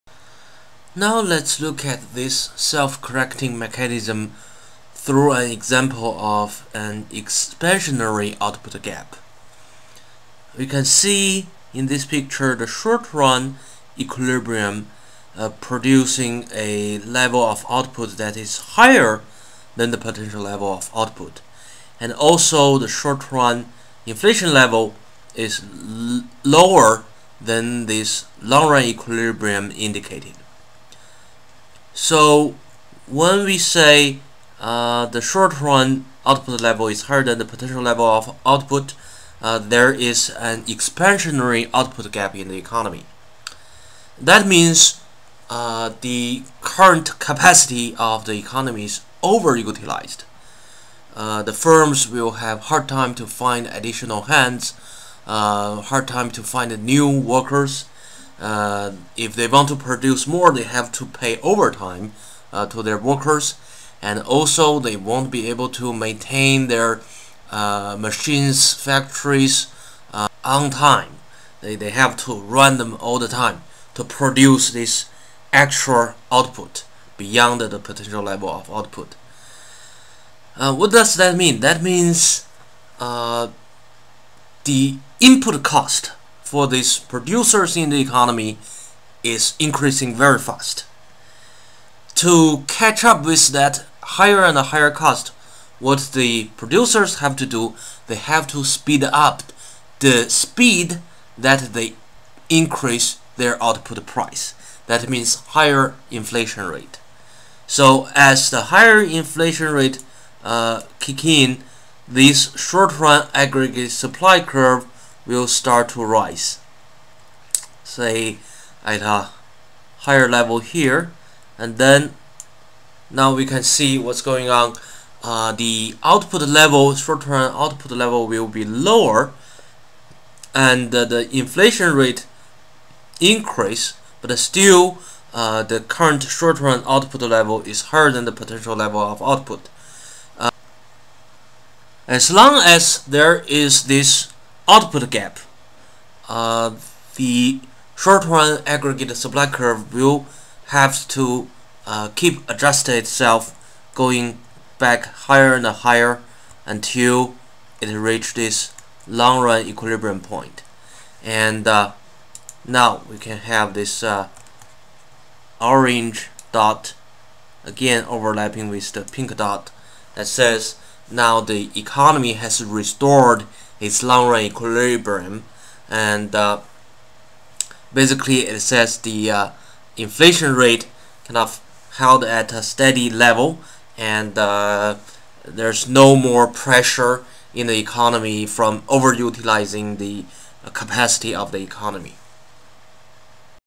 The Self-correcting Mechanism in the AD-AS Model Part 2/2
Demonstrating the self-correcting mechanism of the AD-AS model with an expansionary output gap.